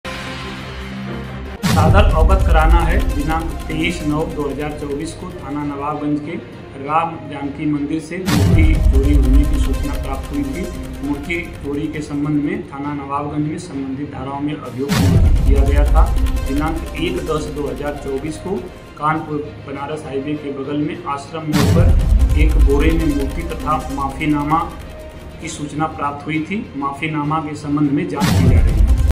मंदिर से चोरी हुई राधा कृष्ण की मूर्ति के वापस मिलने व माफीनामा के सम्बन्ध में सहायक पुलिस आयुक्त
थाना नवाबगंज क्षेत्रान्तर्गत राम जानकी मंदिर से चोरी हुई राधा-कृष्ण की मूर्ति के वापस मिलने व माफीनामा के सम्बन्ध में सहायक पुलिस आयुक्त सोरांव द्वारा दी गई बाइट-*